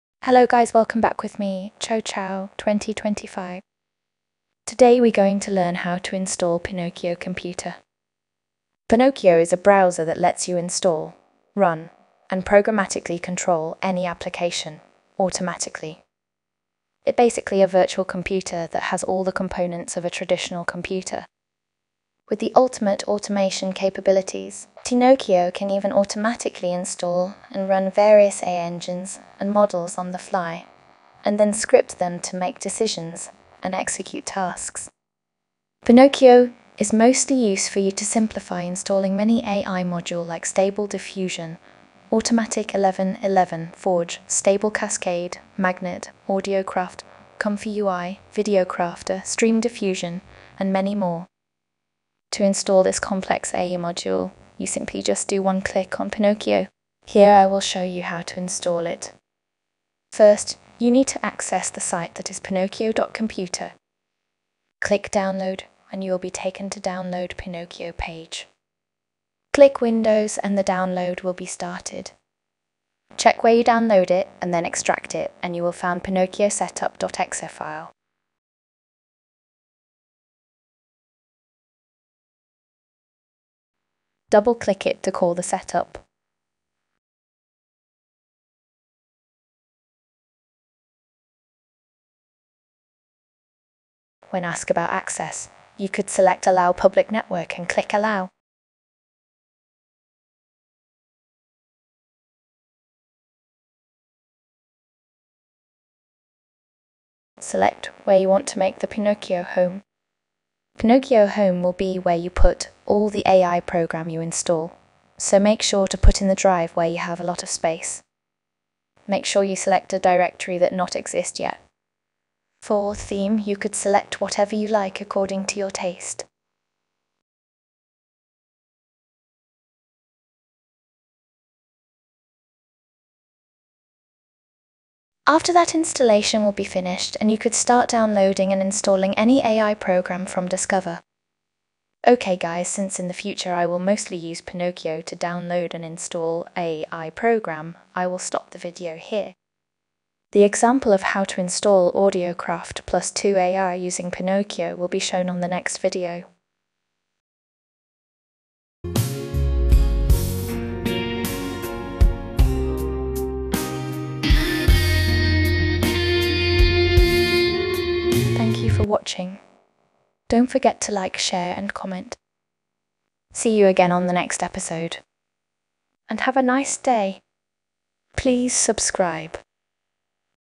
How to Install Pinokio, an app that install, run, and automate any AI applications easily
No more opening the terminal. No more git clone. No more conda install. No more pip install. No more messing with execution environments. Run anything, in a browser.

There are so many applications that require you to open your terminal and enter commands, not to mention deal with all kinds of complicated environment and installation settings.

Pinokio help you simplify all the installation process and help you manage between environment so no more you found non compatible installation. You can install Automatic1111 and Forge stable diffusion, Audiocraft, tortoise, magnet, stable cascade, photomaker and other AI Stuff with one click. The install process will be set up automatically, all you need to do is press okay and wait for it to be over.

With the ultimate automation capabilities, Pinokio can even automatically install and run various AI engines and models on the fly, and then script them to make decisions and execute tasks.

Pinokio is a virtual computer. It has all the components of a traditional computer, except every component is written from scratch to facilitate the main goal, which is to build the ultimate application that can live.